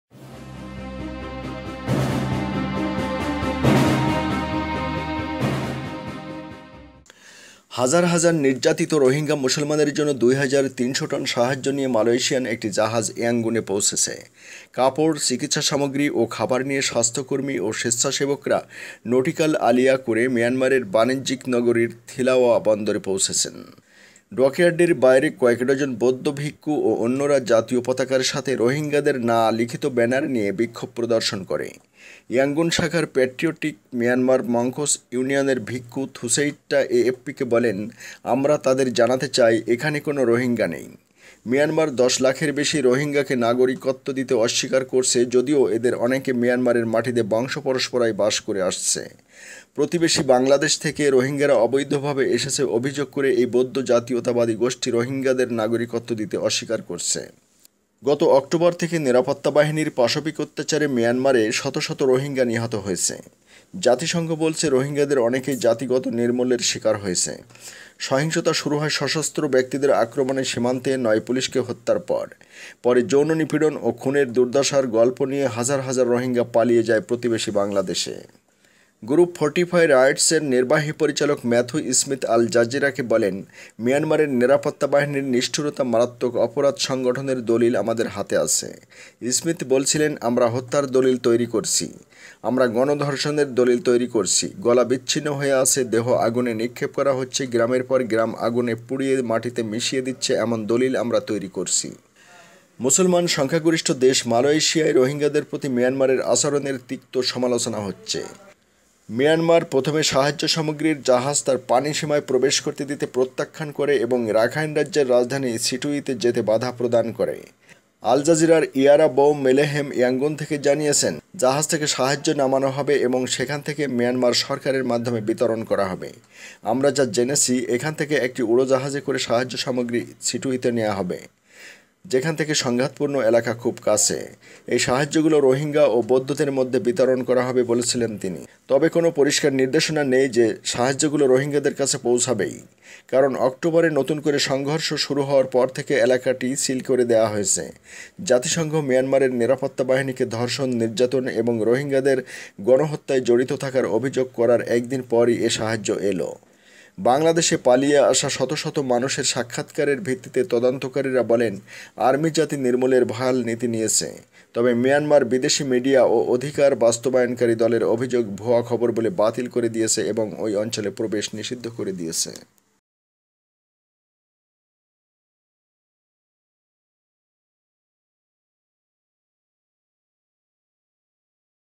২৩০০ টন সাহায্য নিয়ে মিয়ানমারে মালয়েশিয়ান জাহাজ!
২৩০০ টন সাহায্য নিয়ে মিয়ানমারে মালয়েশিয়ান জাহাজ!
২৩০০ টন সাহায্য নিয়ে মিয়ানমারে মালয়েশিয়ান জাহাজ!

হাজার হাজার নির্যাতিত রোহিঙ্গা মুসলমানের জন্য দুই হাজার ৩০০ টন সাহায্য নিয়ে মালয়েশিয়ান একটি জাহাজ ইয়াঙ্গুনে পৌঁছেছে। কাপড়, চিকিৎসা সামগ্রী ও খাবার নিয়ে স্বাস্থ্যকর্মী ও স্বেচ্ছাসেবকরা ‘নটিক্যাল আলীয়া’ করে মিয়ানমারের বাণিজ্যিক নগরীর থিলাওয়া বন্দরে পৌঁছেন। ডকইয়ার্ডের বাইরে কয়েক ডজন বৌদ্ধ ভিক্ষু ও অন্যরা জাতীয় পতাকার সাথে ‘রোহিঙ্গাদের না’ লিখিত ব্যানার নিয়ে বিক্ষোভ প্রদর্শন করে। ইয়াঙ্গুন শাখার প্যাট্রিয়টিক মিয়ানমার মঙ্কস ইউনিয়নের ভিক্ষু থুসেইট্টা এএফপিকে বলেন, ‘আমরা তাদের জানাতে চাই, এখানে কোনো রোহিঙ্গা নেই।’ মিয়ানমার ১০ লাখের বেশি রোহিঙ্গাকে নাগরিকত্ব দিতে অস্বীকার করছে যদিও এদের অনেকে মিয়ানমারের মাটিতে বংশ পরম্পরায় বাস করে আসছে। প্রতিবেশী বাংলাদেশ থেকে রোহিঙ্গারা অবৈধভাবে এসেছে অভিযোগ করে এই বৌদ্ধ জাতীয়তাবাদী গোষ্ঠী রোহিঙ্গাদের নাগরিকত্ব দিতে অস্বীকার করছে।